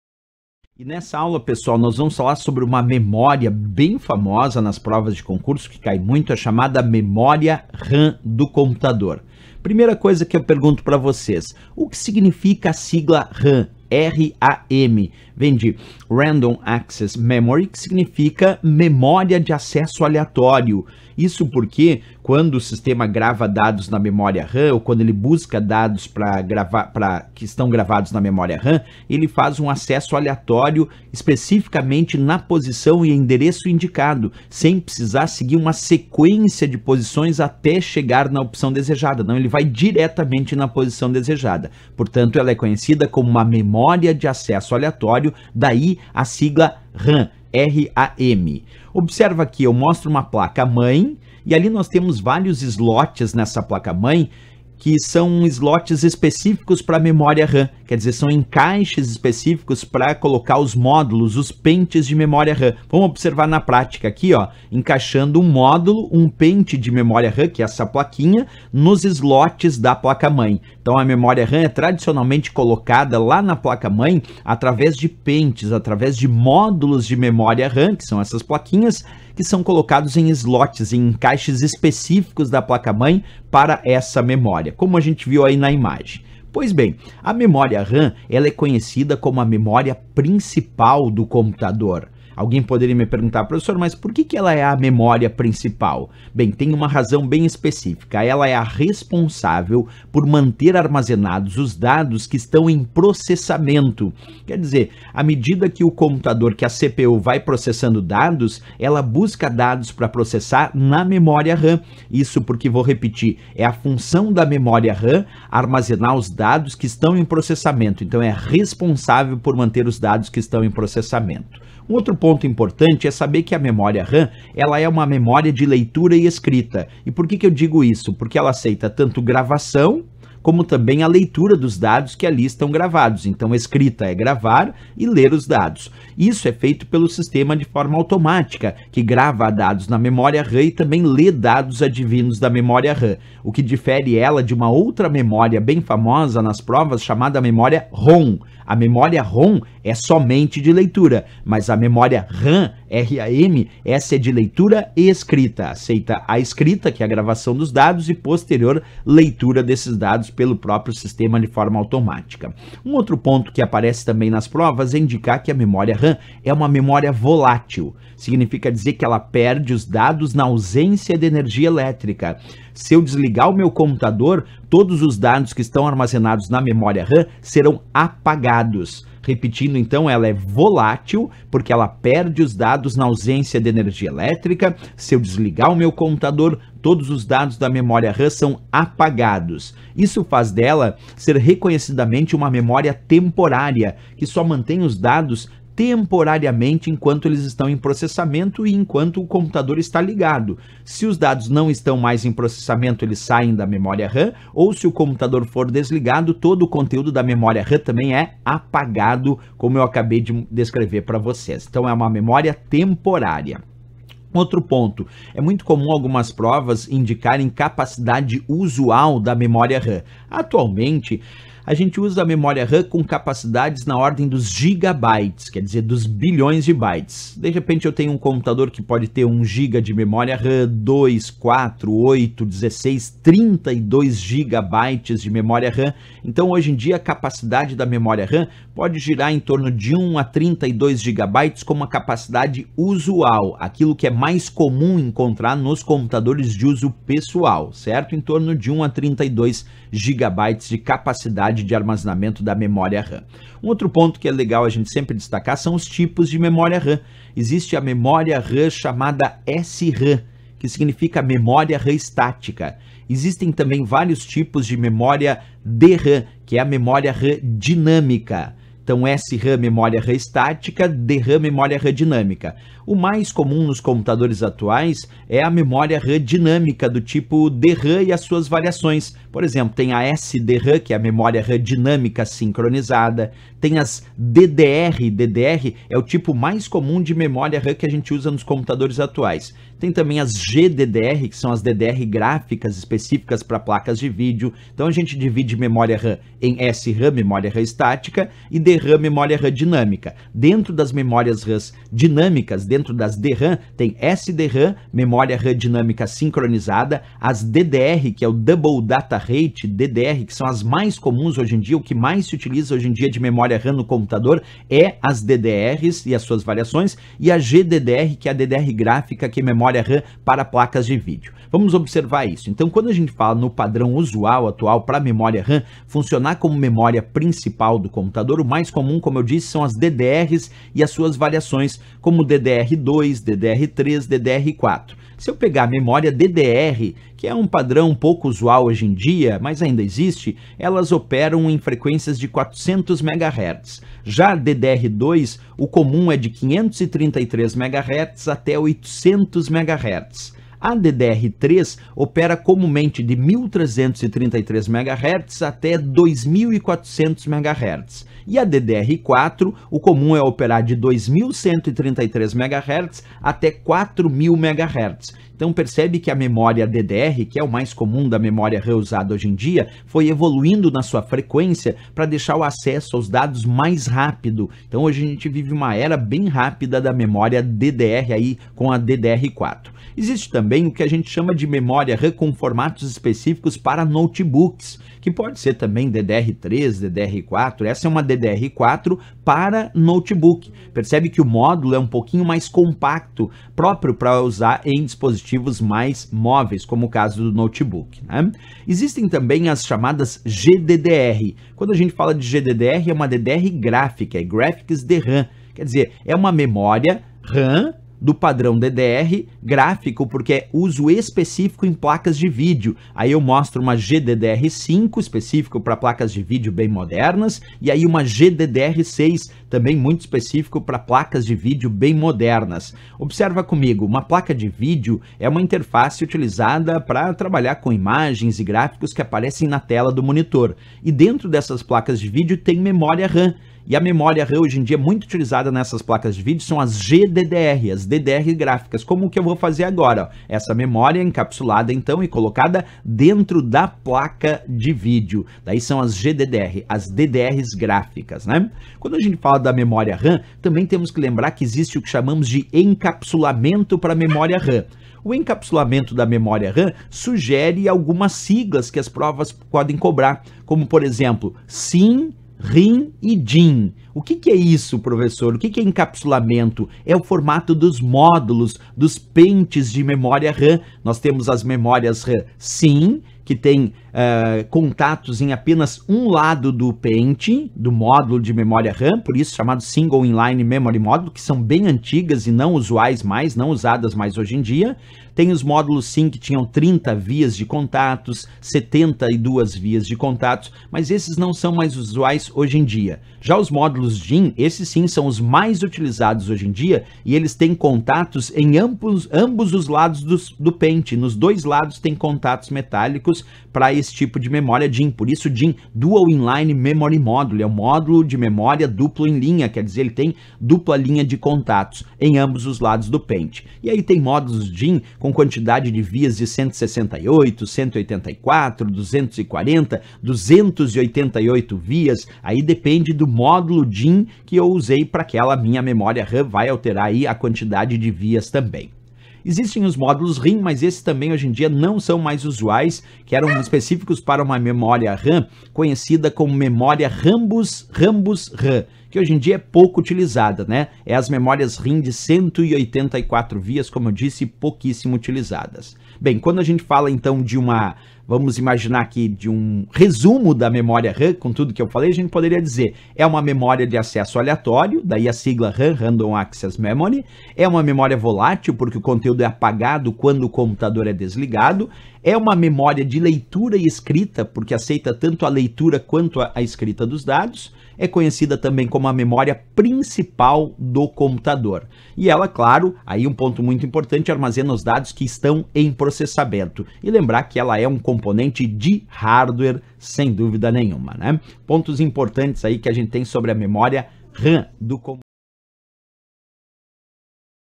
Memória RAM
No computador a memória RAM exerce a função de memória principal. Conheça agora os detalhes desta memória com o professor Rodrigo Schaeffer.